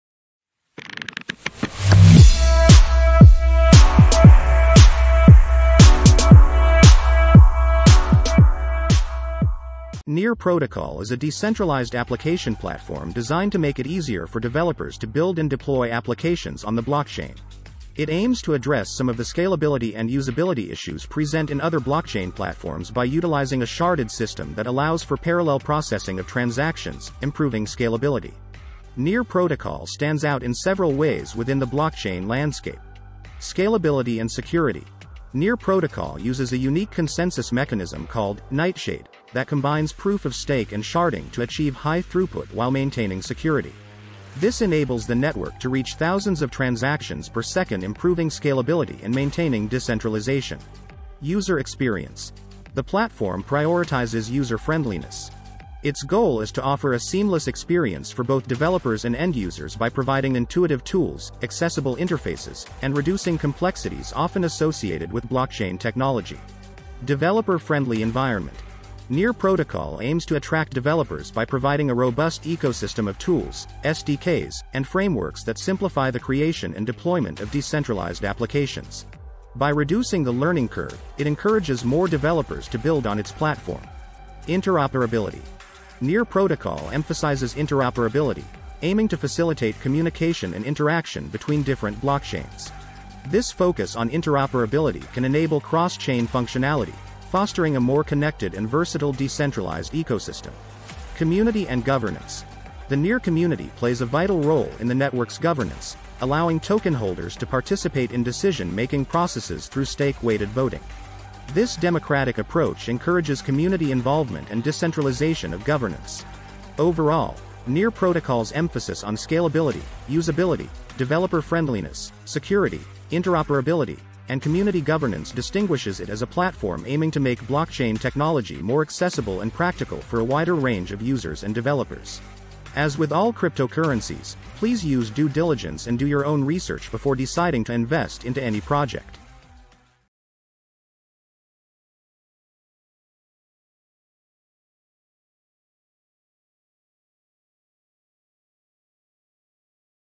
NEAR Protocol - The Future of Blockchain?
A quick review of the NEAR Protocol and why I think it's the future of blockchain. Scalable, quicker, secure, and user friendly for the developers and users. You decide.